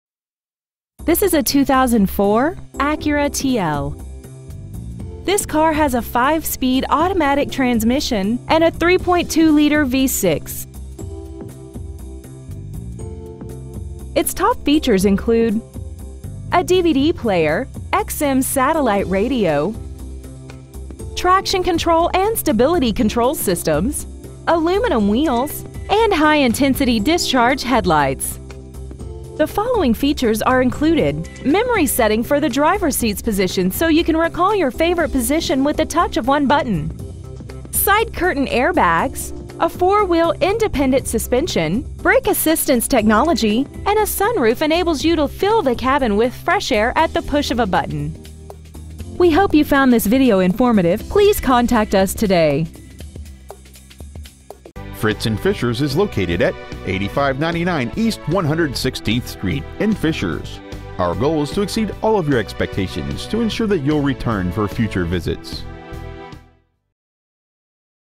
2004 Acura TL Fishers IN
We've been honored to serve the Fishers IN area , we promise that your experience at our dealership will exceed your expectations ! Year : 2004 Make : Acura Model : TL Engine : 3.2L V6 24V MPFI SOHC Trans . : 5-Speed Automatic Exterior : Gray Miles : 112,537 Interior : Ebony Stock : 1094 Fritz in Fishers East 116th Street Fishers , IN 46038